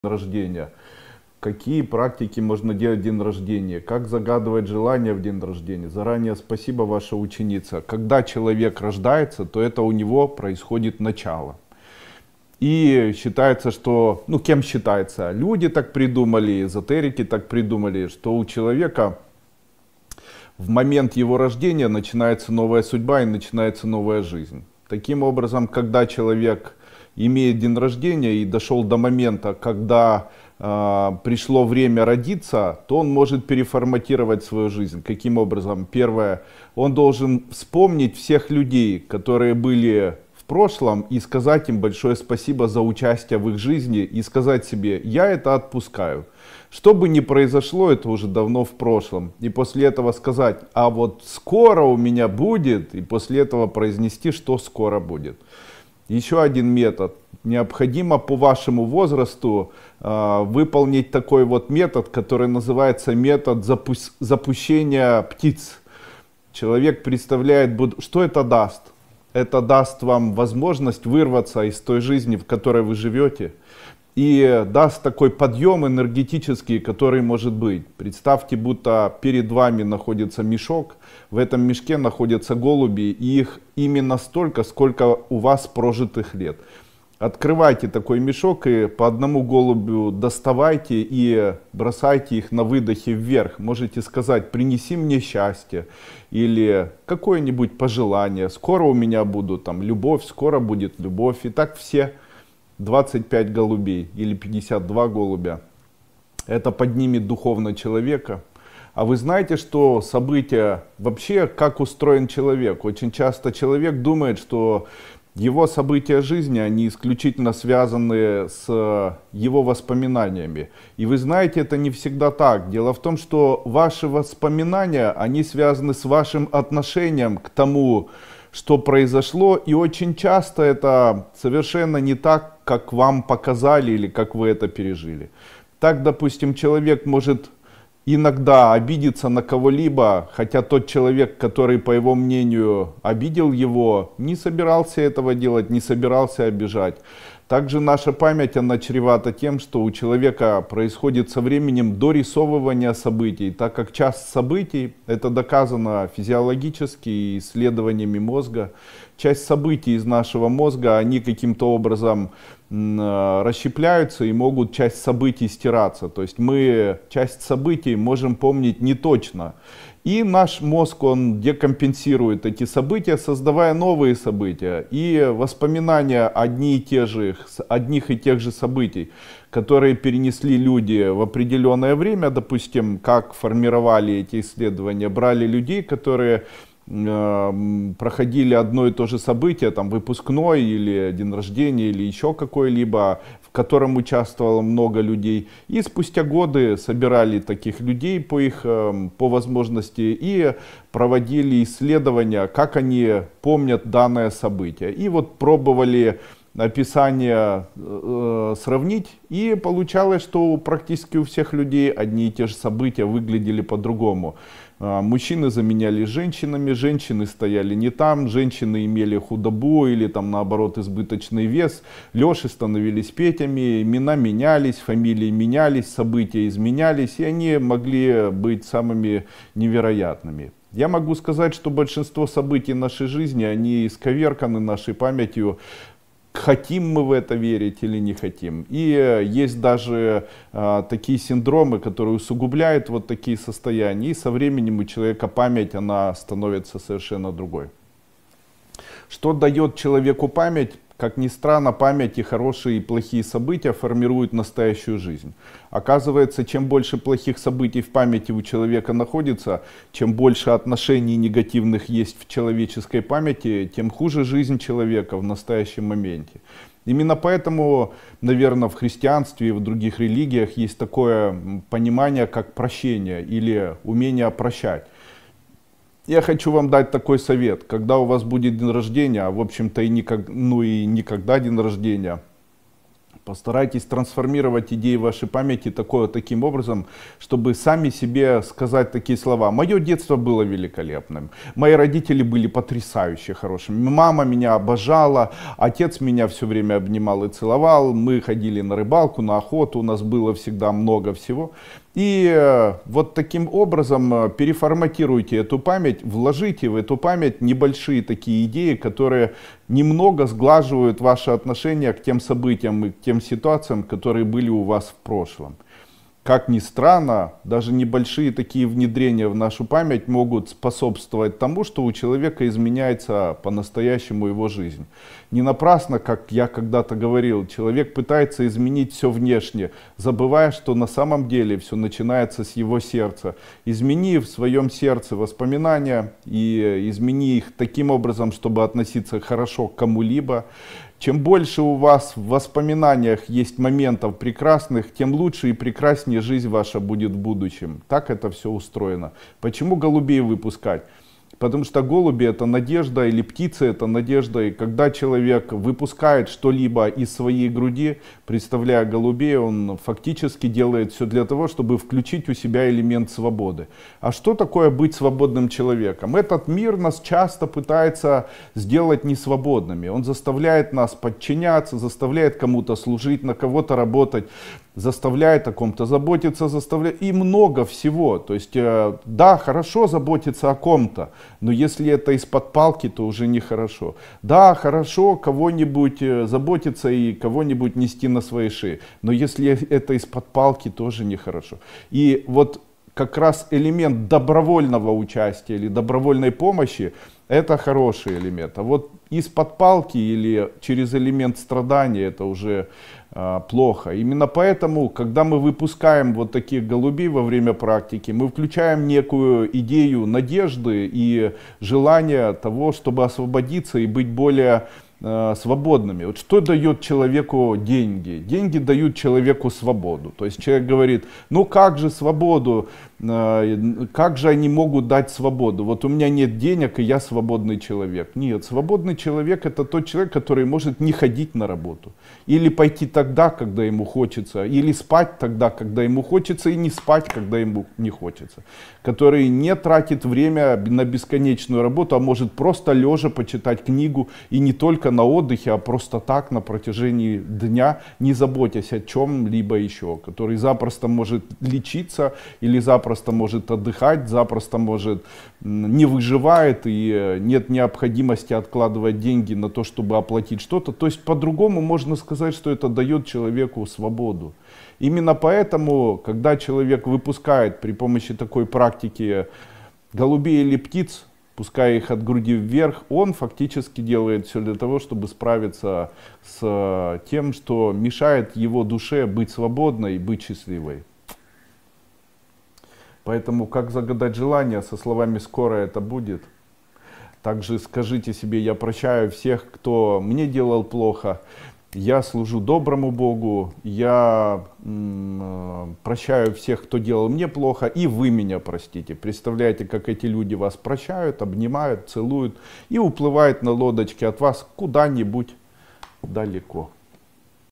➣Как День Рождения Становится Стартом К Исполнению Желаний?
В этом видео мы обсудим значимость Дня Рождения как начала нового этапа и методы загадывания желаний в этот особенный день. Рассмотрим, какие практики помогают сбыванию желаний и почему важно избегать воспоминаний о негативных событиях, чтобы не привлекать их в будущее.